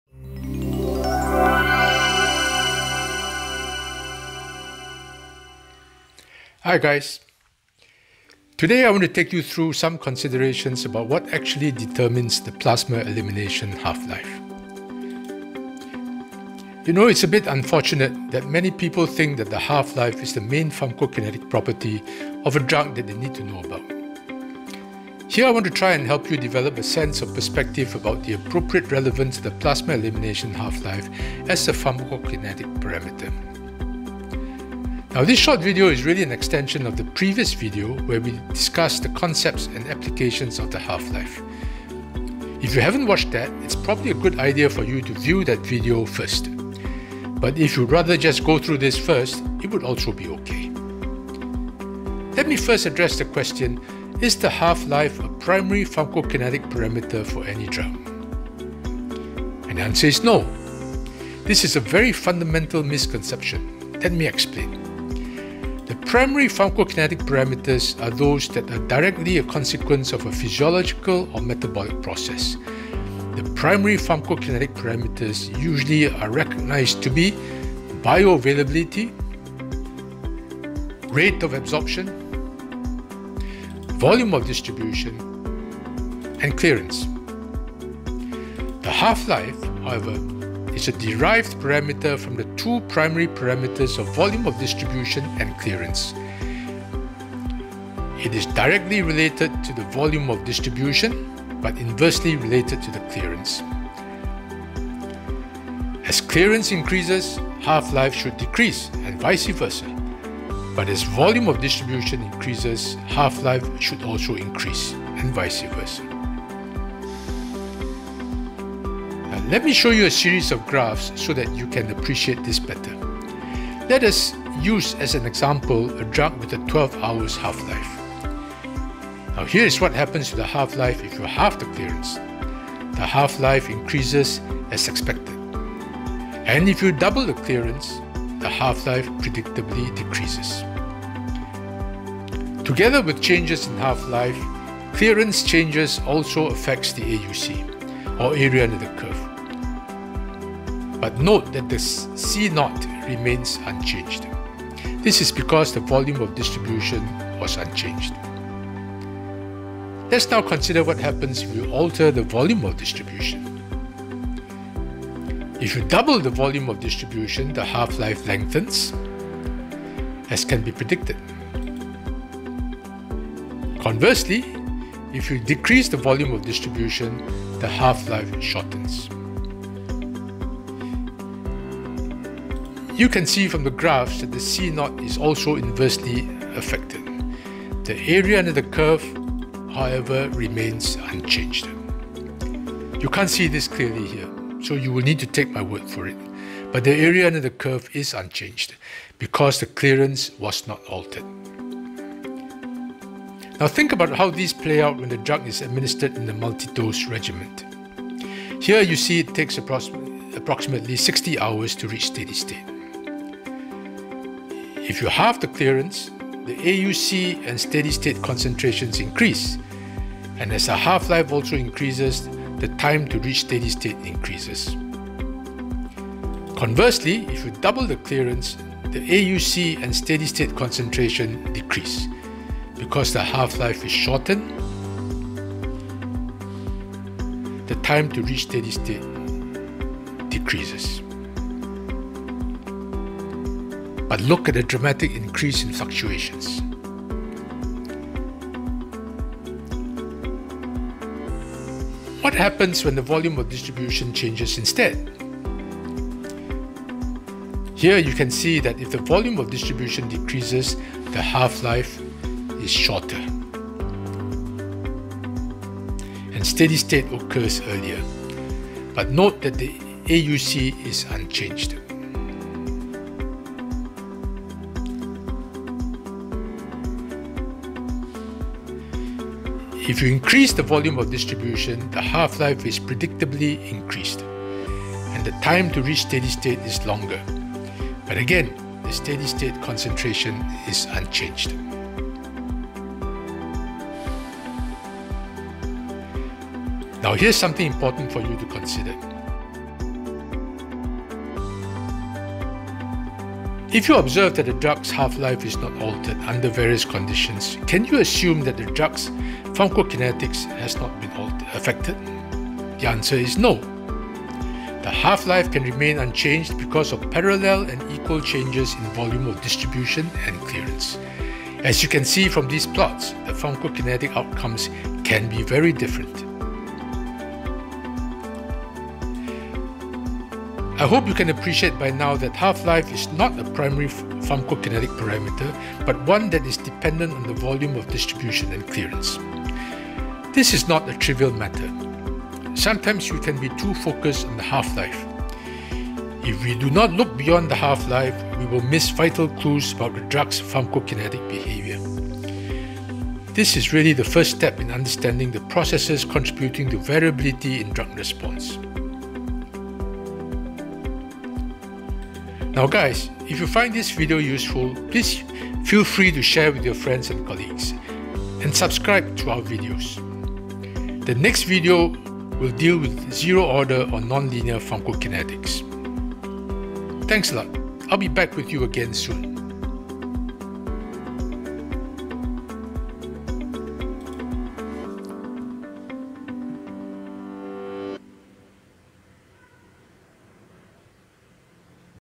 The Pharmacokinetics series: Determinants of Elimination Half-life
It is a bit unfortunate that many people think the half-life is the main pharmacokinetic property of a drug they need to know about.
Here I want to try and help you develop a sense of perspective about the appropriate relevance of the plasma elimination half-life as a pharmacokinetic parameter.

This short video is really an extension of the previous video where we discussed the concepts and applications of the half-life. If you haven’t watched that, it is probably a good idea for you to view that video first. But if you would rather just go through this first, it would also be ok.

Let me first address the question, “Is the half-life a primary pharmacokinetic parameter for any drug”? The answer is “NO”. This is a very fundamental misconception. Let me explain.

The primary pharmacokinetic parameters are those that are directly a consequence of a physiological or metabolic process. The primary pharmacokinetic parameters are recognized to be –bioavailability, ….. rate of absorption,…. volume of distribution,….. and clearance.

The half-life, however, is a derived parameter from the 2 primary parameters of volume of distribution and clearance. It is directly related to the volume of distribution, but inversely related to the clearance. As clearance increases, half-life should decrease…. And vice-versa. But as volume of distribution increases, half-life should also increase, … and vice versa.

Let me show you a series of graphs so that you can appreciate this better.

Let’s use as an example a drug, with a 12 hours half-life.
Here is what happens to the half-life if you halve the clearance. The half-life increases as expected. If you double the clearance, the half-life predictably decreases. Together with changes in half-life, clearance changes also affect the AUC, or area under the curve. But, note that the C0 remains unchanged. This is because the volume of distribution was unchanged.

Let’s now consider what happens if you alter the volume of distribution.
If you double the volume of distribution, the half-life lengthens as can be predicted. Conversely, if you decrease the volume of distribution, the half-life shortens. You can see from the graphs that the C0 is also inversely affected. The area under the curve, however, remains unchanged. You can’t this clearly here, so you will need to take my word for it. The AUC is unchanged…. because, the clearance was not altered.

Think about how these play out when the drug is administered in a multi-dose regiment.

Here you see it takes approximately 60 hours to reach steady state.

If you halve the clearance, the AUC and steady state concentrations increase, and as the half-life also increases, the time to reach steady state increases.
Conversely, if you double the clearance, the AUC and steady state concentration decrease. Because the half-life is shortened, the time to reach steady state is decreased.
And look at the dramatic increase in fluctuations.
What happens when the volume of distribution changes instead? Here you can see that if the volume of distribution decreases, the half-life is shorter and steady state occurs earlier. But note that the AUC is unchanged!

If you increase the volume of distribution, the half-life is predictably increased, and the time to reach steady state is longer. But again, the steady state concentration is unchanged.
Now, here’s something important for you to consider. If you observe that a drug’s half-life is not altered under various conditions, can you assume that the drug’s pharmacokinetics has not been affected?

The answer is no. The half-life can remain unchanged because of parallel and equal changes in volume of distribution and clearance. As you can see from these plots, the pharmacokinetic outcomes can be very different.

I hope you can appreciate by now that half-life is not a primary pharmacokinetic parameter but one that is dependent on the volume of distribution and clearance. This is not a trivial matter. Sometimes, we can be too focused on the half-life. If we do not look beyond the half-life, we will miss vital clues about the drug’s pharmacokinetic behavior. This is the first step in understanding the processes contributing to variability in drug response.